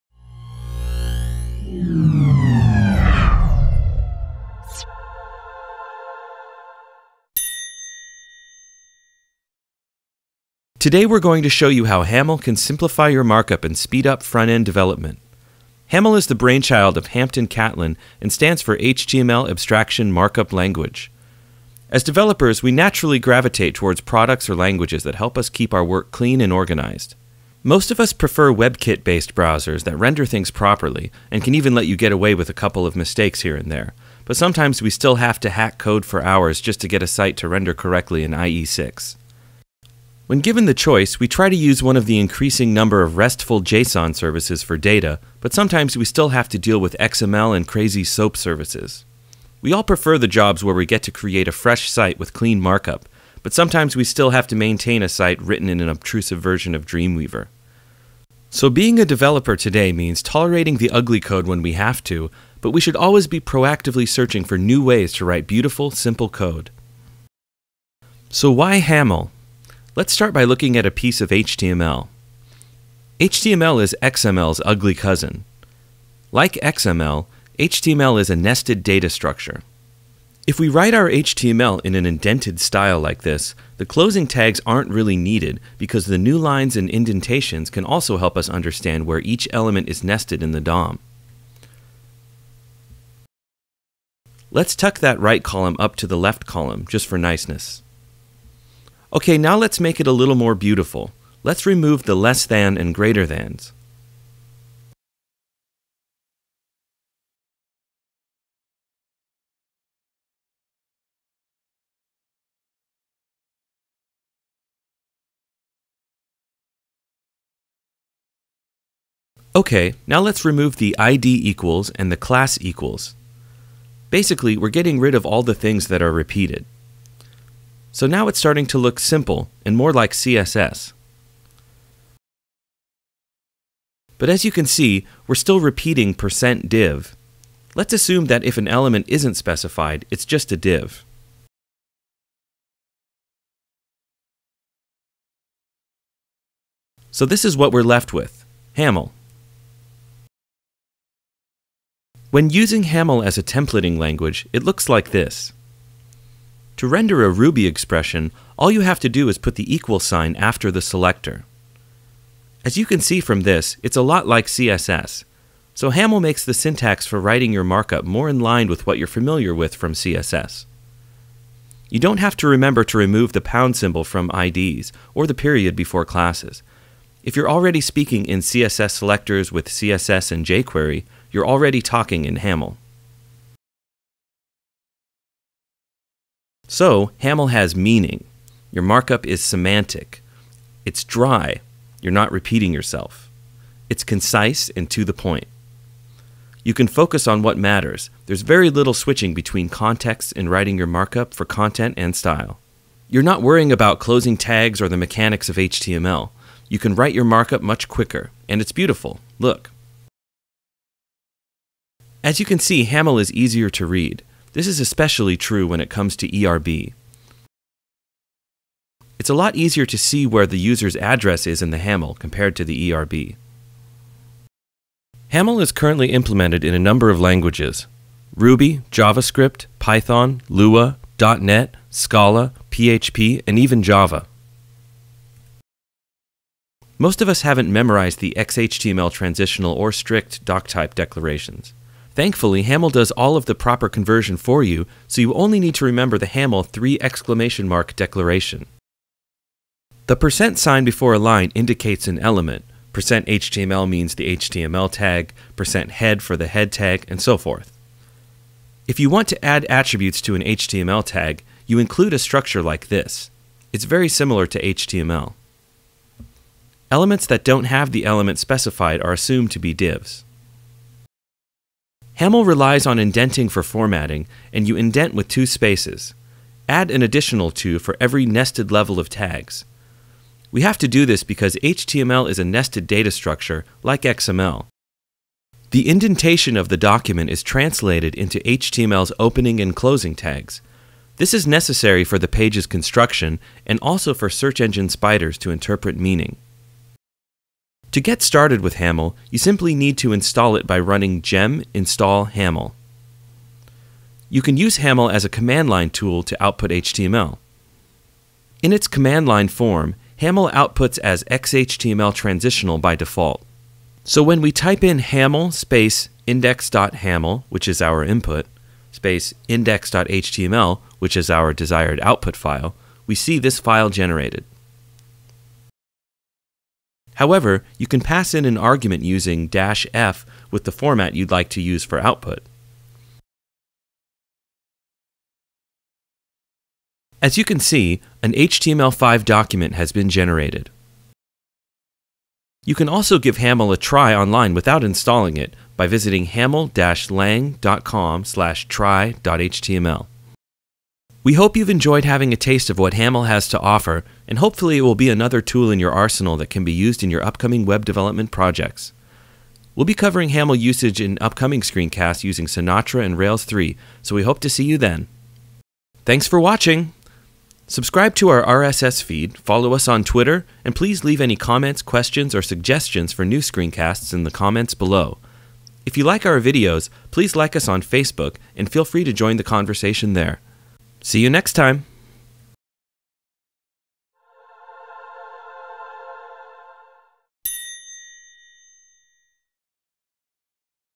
Introduction to Haml
Haml is the brain child of Hampton Catlin. In this screencast we're going to show how Haml can simplify your markup and speed up front-end development. As developers we naturally gravitate towards products or languages that help keep our work clean and organized. Haml is just one of those things. Watch this screencast to see why!

Created by ANDREW CHALKLEY  JOSH TIMONEN